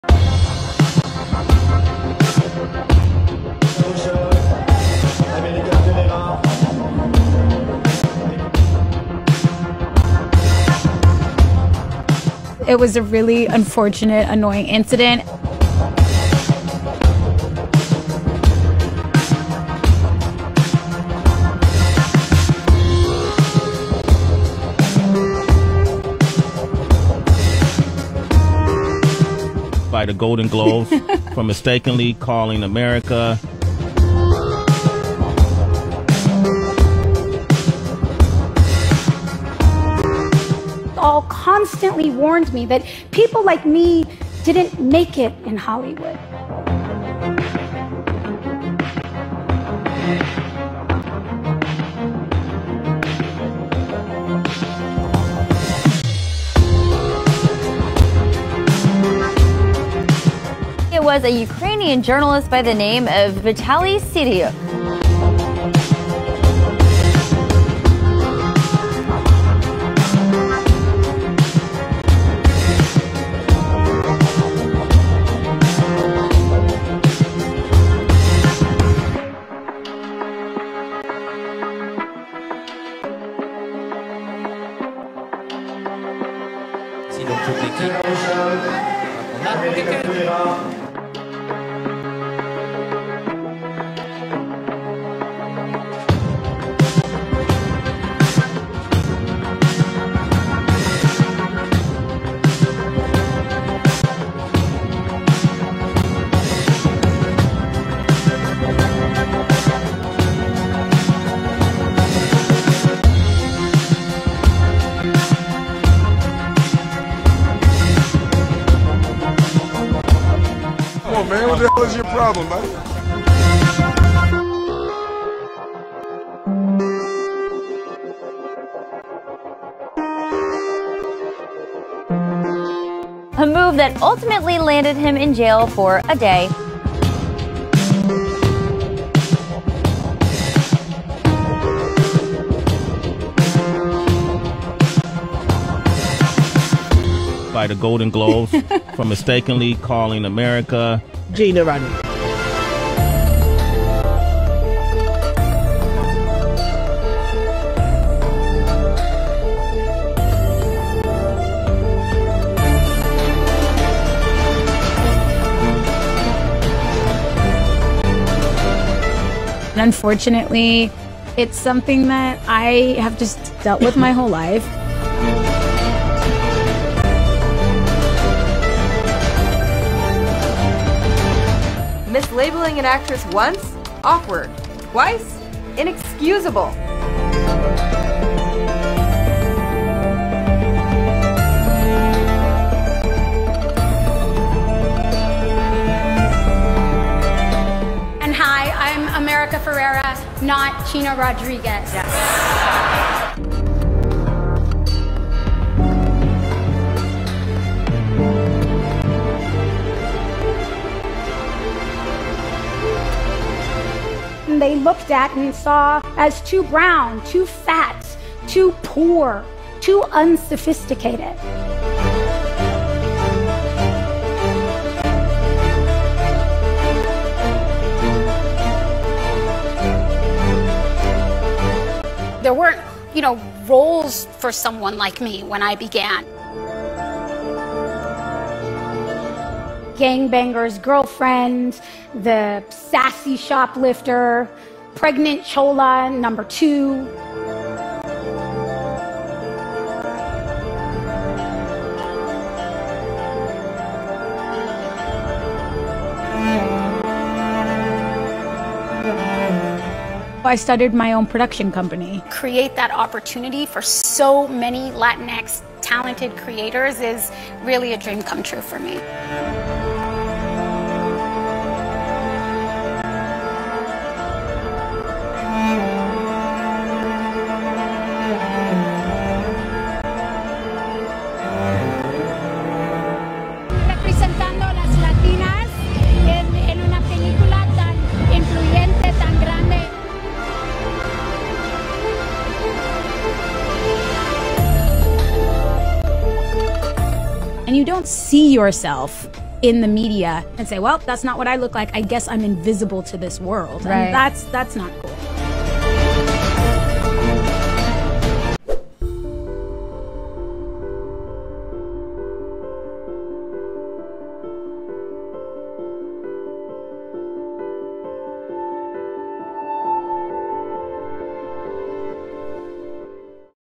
"Barbie" and "Ugly Betty" actress is denigrated on the red carpet
America Ferrera, who now shines for her role in Barbie, fought hard to make a place for herself in Hollywood, but there were still people who did not respect her, and at the Cannes Film Festival, she ended up being publicly humiliated.